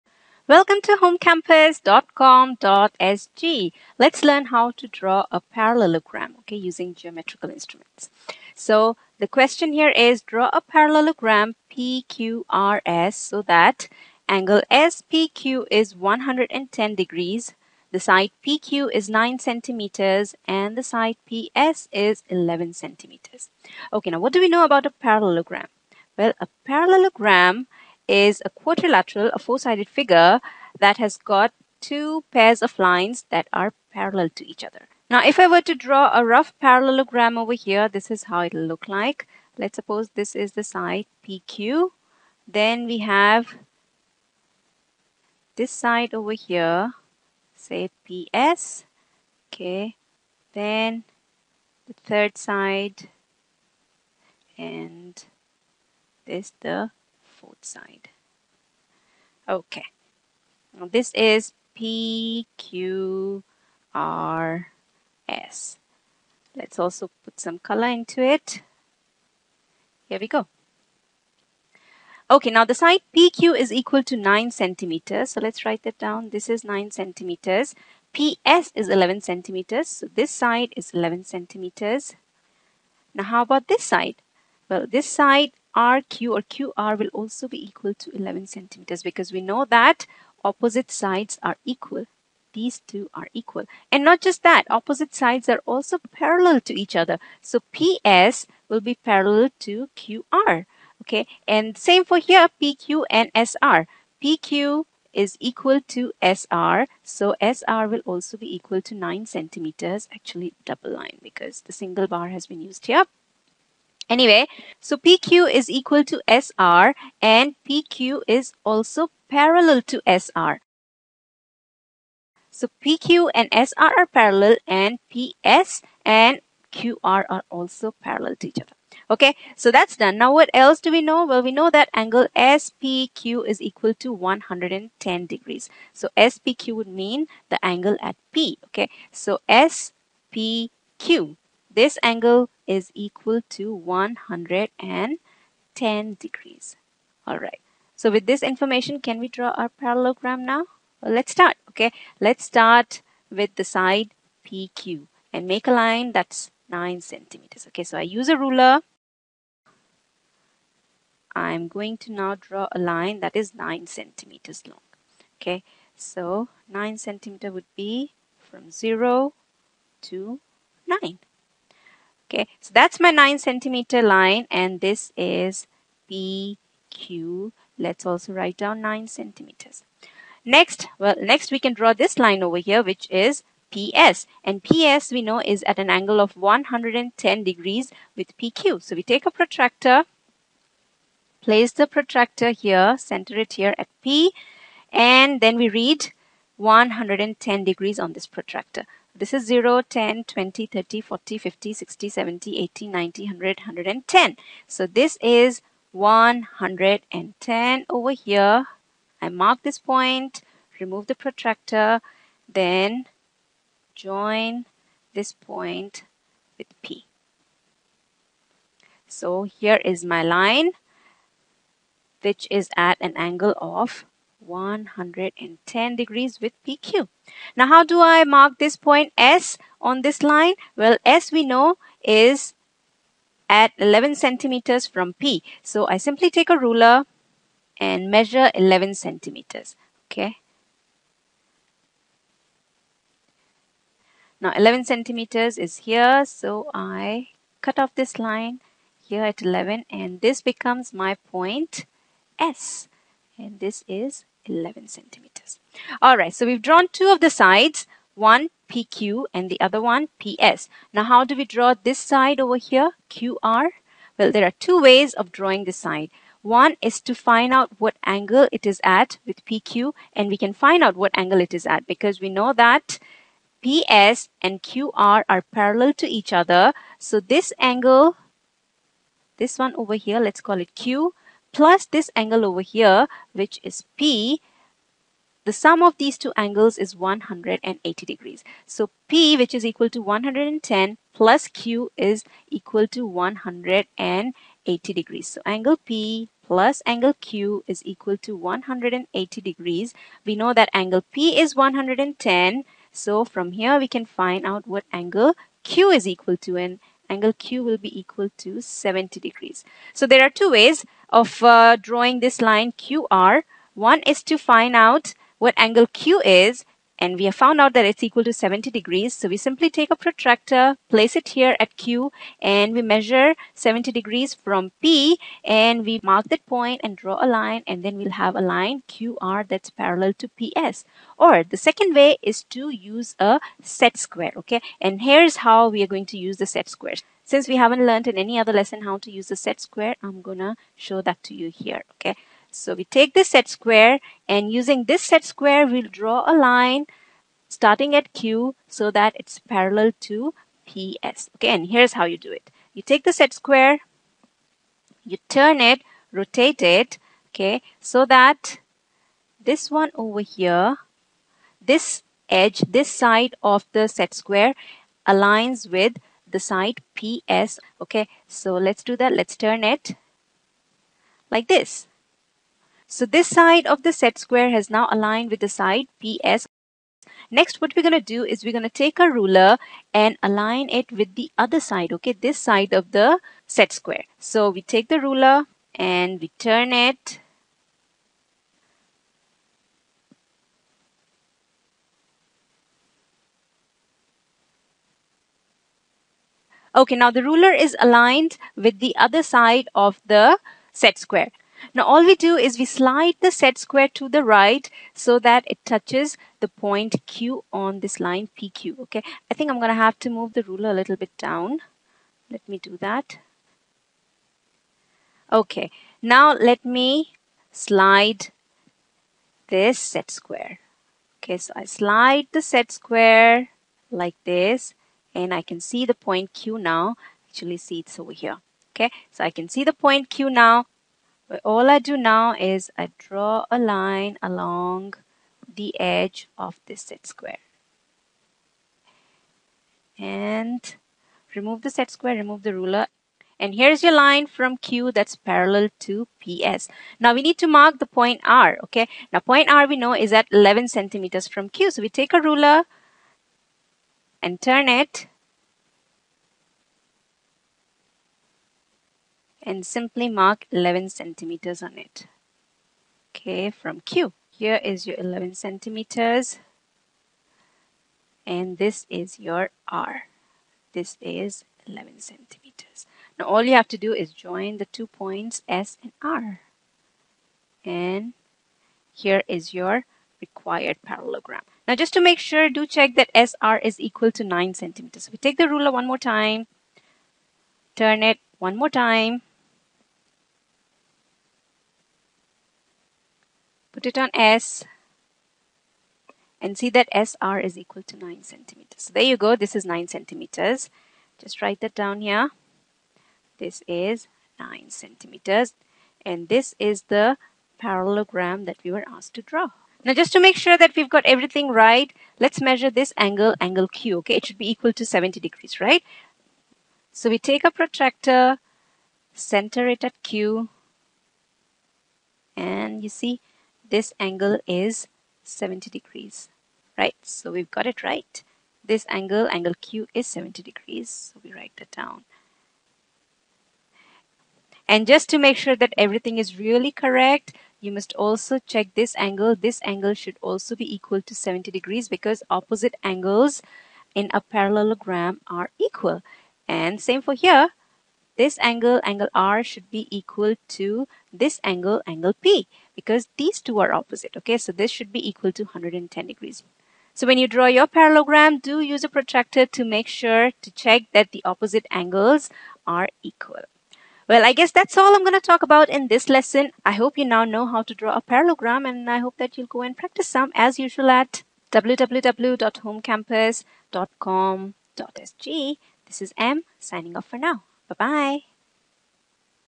Singapore Math, Grade 5 / Primary 5: Geometry - Drawing parallelogram using instruments
: Learn how to draw a parallelogram in a few easy steps.